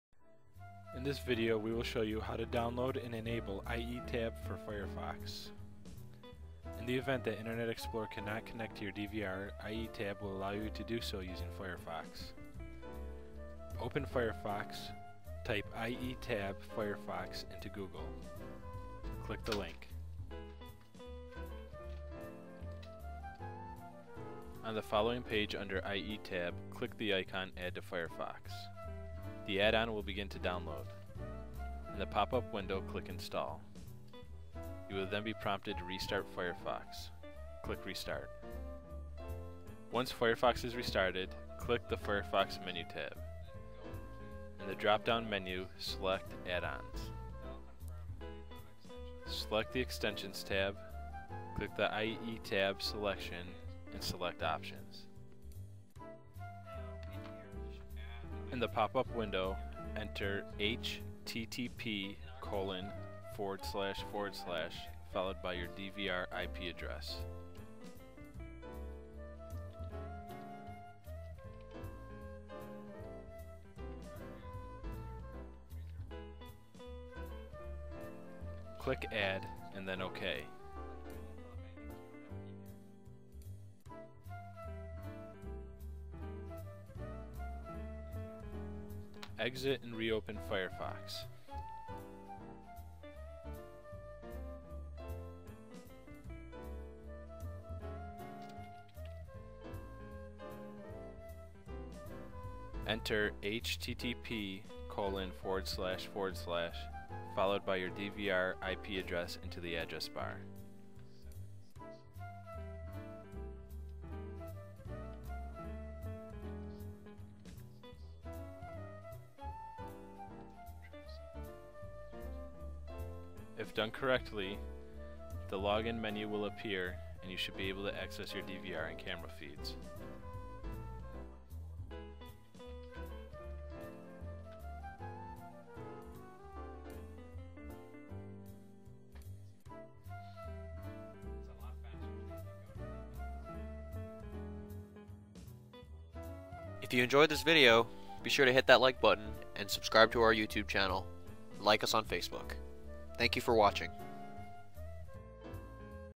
Download and enable IE Tab for Firefox
In this video we will show you how to download and enable IE Tab for Firefox.  This can be done if you are unable to access your DVR using Internet Explorer.